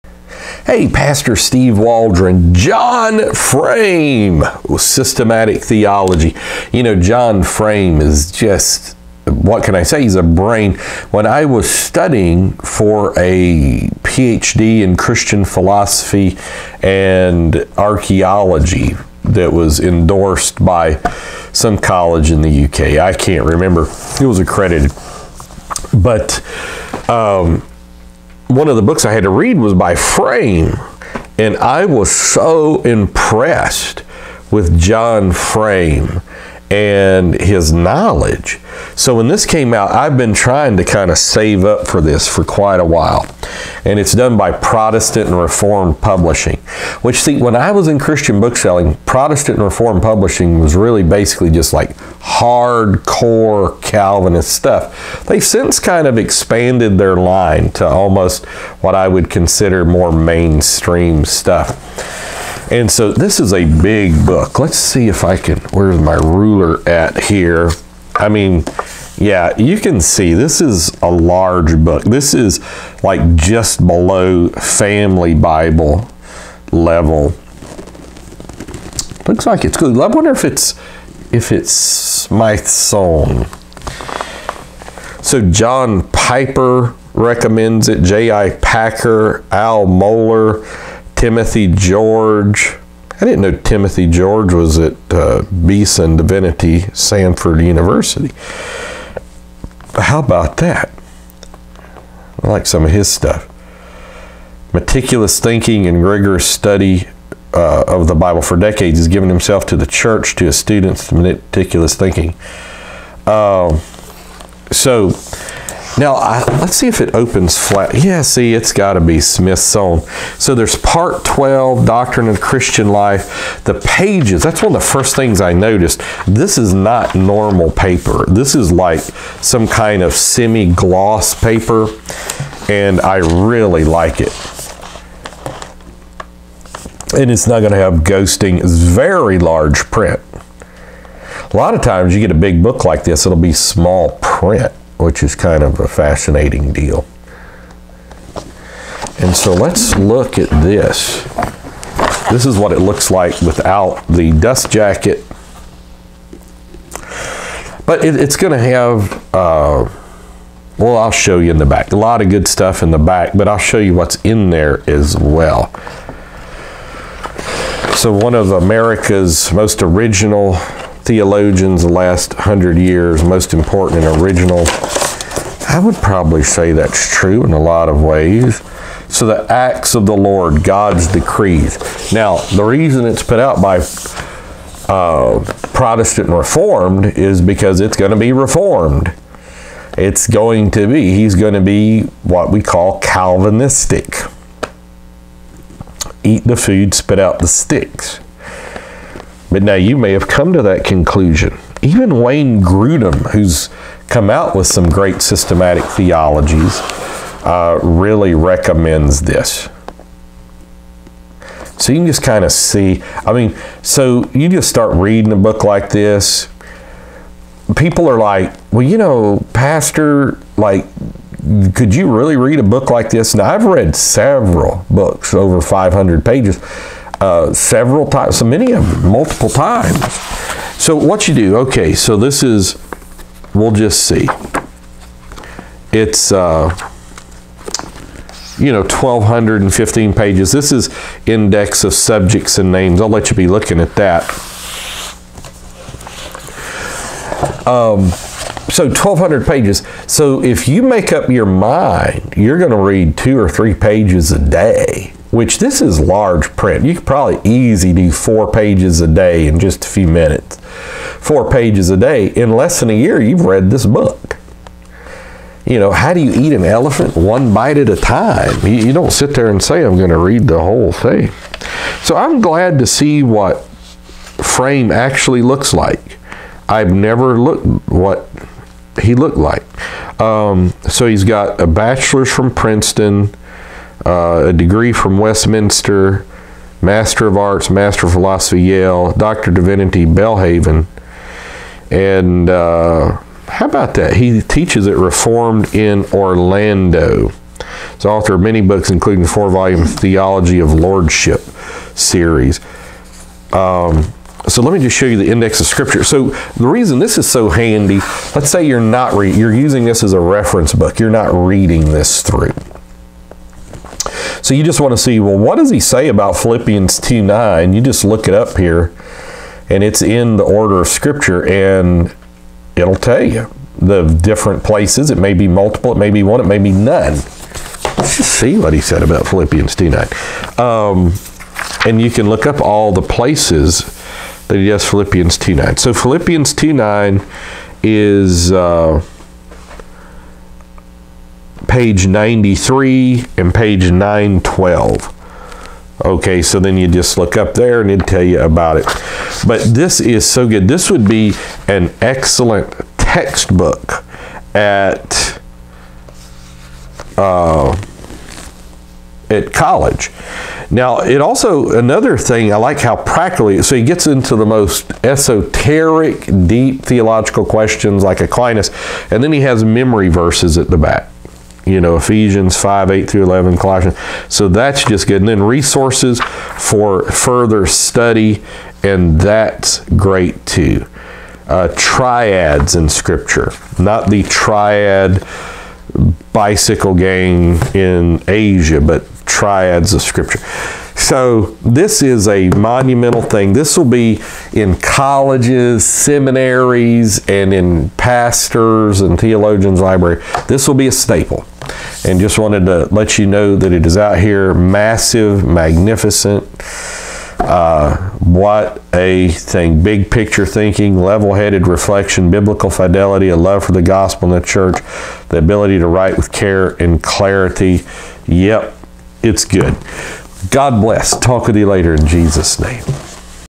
Systematic Theology By John Frame l Review
An outstanding Systematic Theology by one of my favorite authors!  From P&R Publishers.  To contribute to New Life, please click the link below and press donate. God bless!